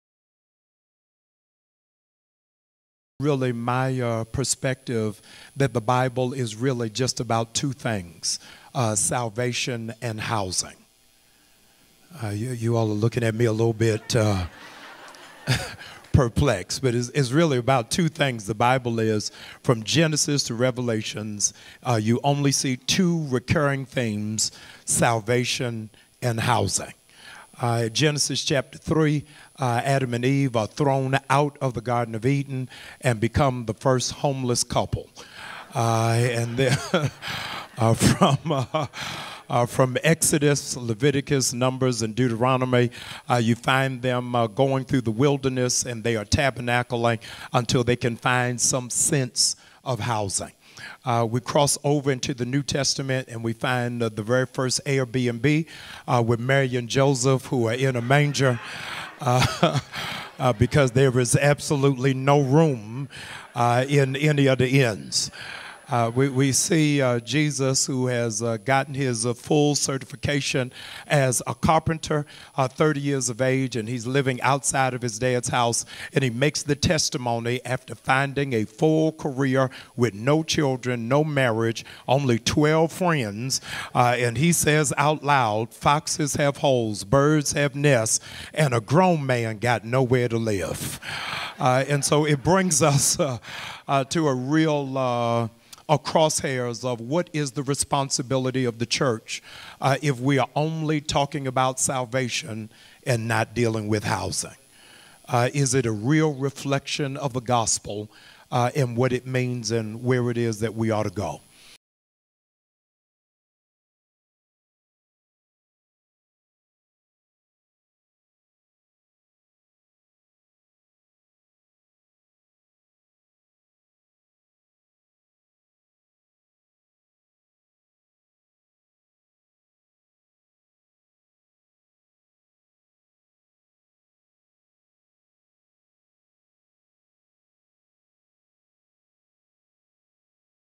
"The Bible Is About Two Things Salvation And Housing"
In this clip, Pastor Jamal Bryant' makes a statement that "the Bible is about two things: salvation and housing". Check it out and share your thoughts.